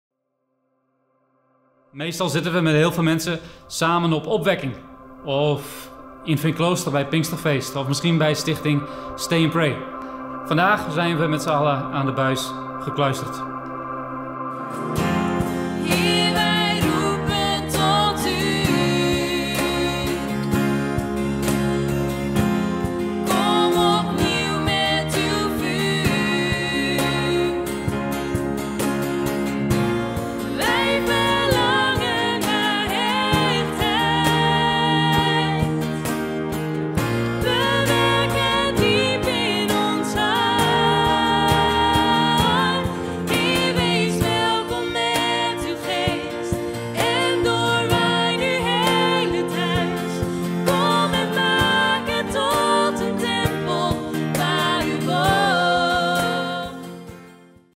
Nehemia Dokkum online dienst op Pinksteren 📺 Kijk je mee?
Kijk jij aankomende zondag 31 mei om 10.00 uur mee naar de online dienst van Nehemia Dokkum? 📺

Wil je op de hoogte blijven van alle video's van Nehemia?

 📱 Al het laatste nieuws van Nehemia vind je in de Scipio app. Download 'm nu in de App Store of Play Store!